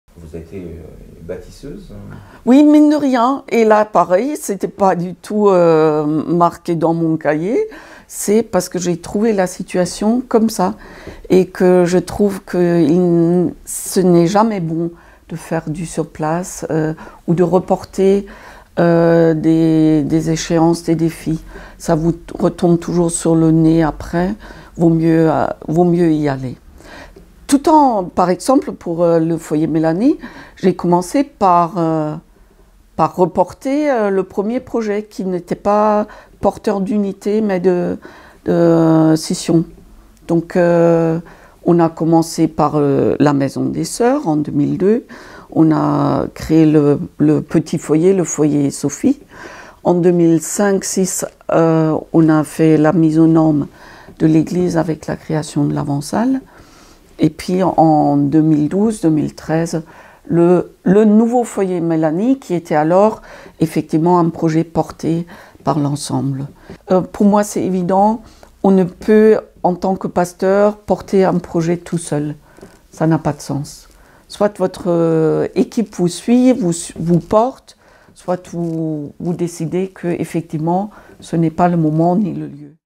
4/7 : Monika Garruchet, le nouveau foyer Mélanie de Pourtales.
4/7 : Monika Garruchet, le nouveau foyer Mélanie de Pourtales.

Rencontre avec la Pasteure Monika Garruchet à la veille de son départ. Après 16 années de présence à la paroisse Protestante Robertsau, le Blog de la Robertsau a rencontré cette figure du quartier.